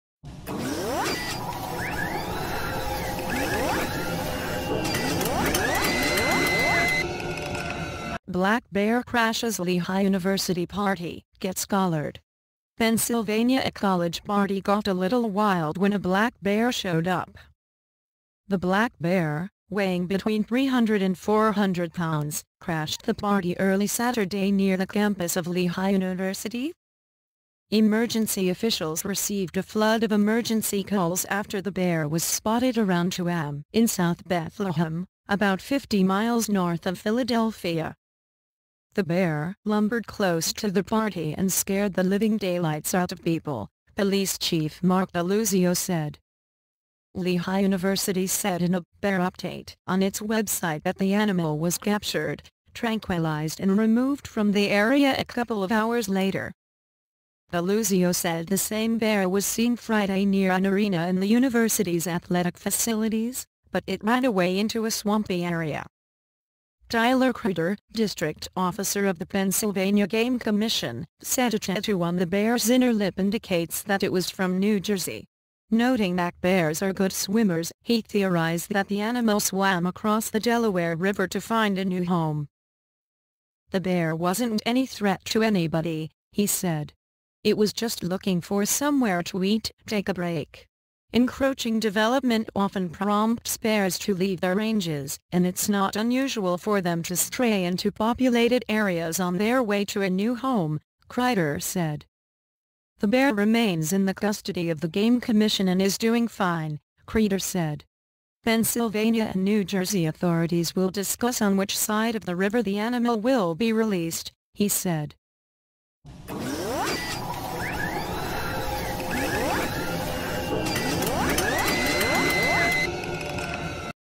bear crashes Lehigh University party
Black bear crashes Lehigh University party, gets collared
Pennsylvania –  A college party got a little wild when a black bear showed up.
The black bear, weighing between 300 and 400 pounds, crashed the party early Saturday near the campus of Lehigh University.

Emergency officials received a flood of emergency calls after the bear was spotted around 2 a.m. in south Bethlehem, about 50 miles north of Philadelphia.
The bear "lumbered close to the party and scared the living daylights out of people," police Chief Mark DiLuzio said.
Lehigh University said in a "Bear Update" on its website that the animal was captured, tranquilized and removed from the area a couple of hours later.

DiLuzio said the same bear was seen Friday near an arena and the university's athletic facilities, but it ran away into a swampy area.
Tyler Krieder, district officer of the Pennsylvania Game Commission, said a tattoo on the bear's inner lip indicates that it was from New Jersey. Noting that bears are good swimmers, he theorized that the animal swam across the Delaware River to find a new home.

"The bear wasn't any threat to anybody," he said. "It was just looking for somewhere to eat, take a break."
Encroaching development often prompts bears to leave their ranges, and it's not unusual for them to stray into populated areas on their way to a new home, Kreider said.
The bear remains in the custody of the game commission and is doing fine, Krieder said. Pennsylvania and New Jersey authorities will discuss on which side of the river the animal will be released, he said.